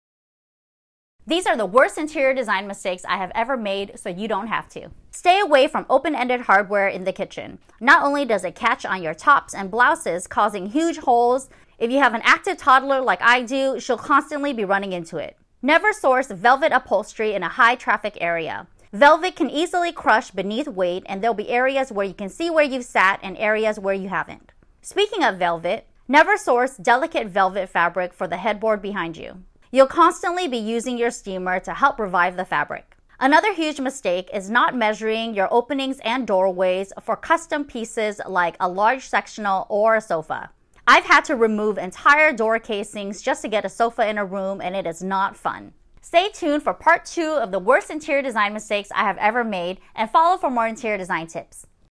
Worst Interior Design Mistakes I've Made | Pt 1 of 2
Designers, they’re just like us! 😆 Happy to report that I’ve since swapped out these cabinet handles for close-ended ones! 👍 Tell me, what the worst interiordesignmistake you have made in your home?